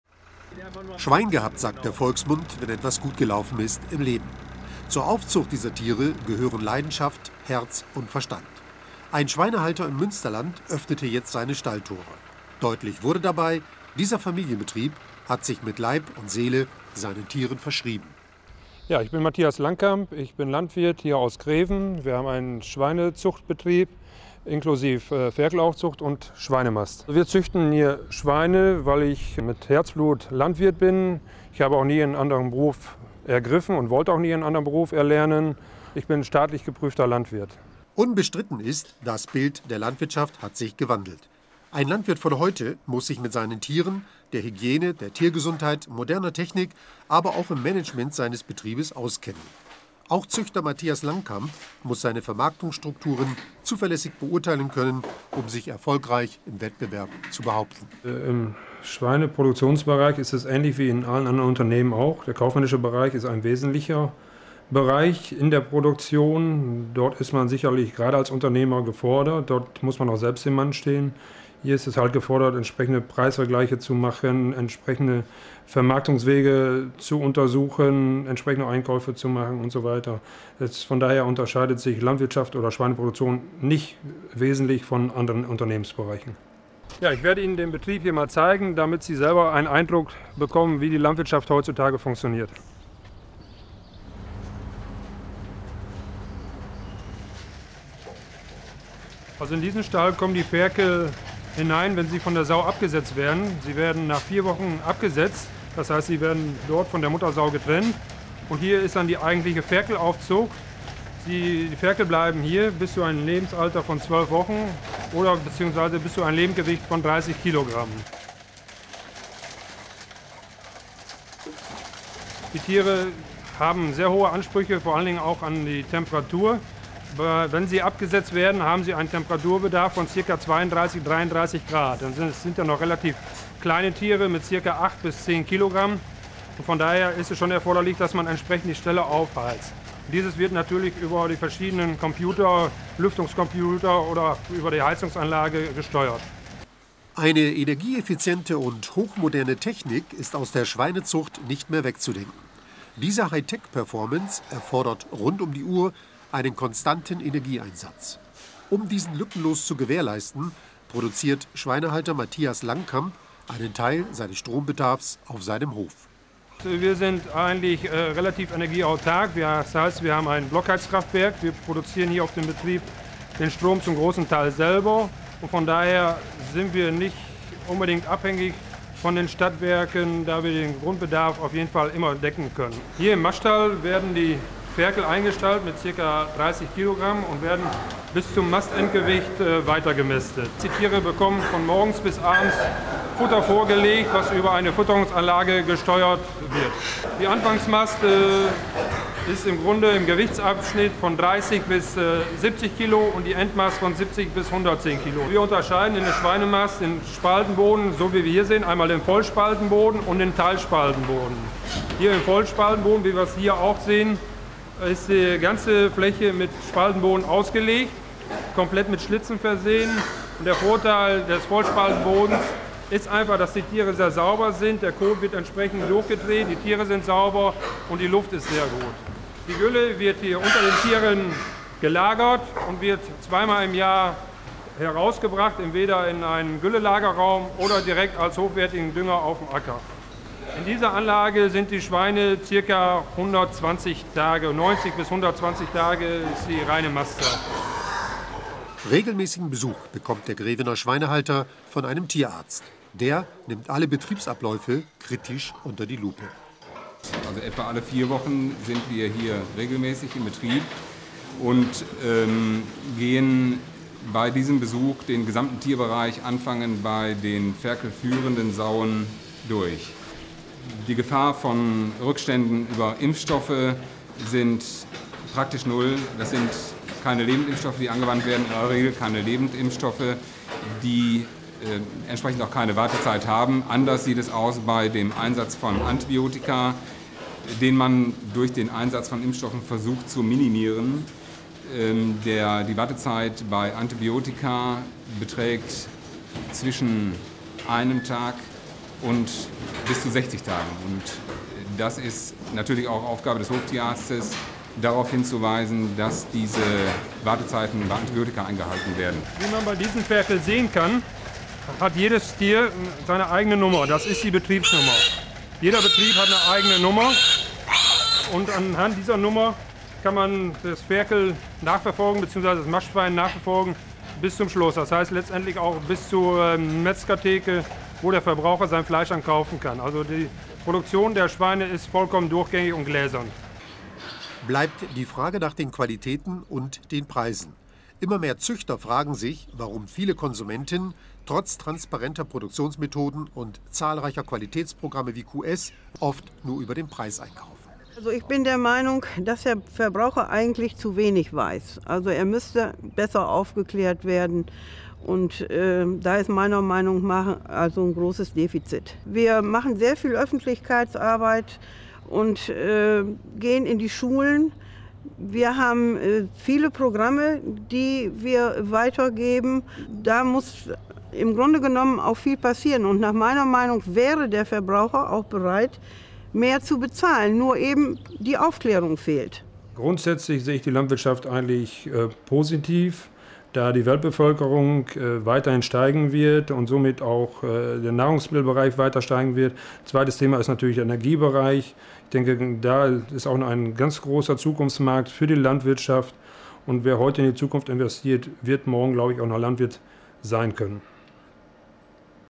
Moderne Schweinehaltung im Münsterland
Matthias Langkamp aus Greven (Nordrhein-Westfalen) stellt seinen Betrieb vor und berichtet über seinen Alltag als Schweine haltender Bauer.  Im Film des Westfälisch-Lippischen Landwirtschaftsverbandes gewährt er Einblick in seine tägliche Arbeit mit den Tieren.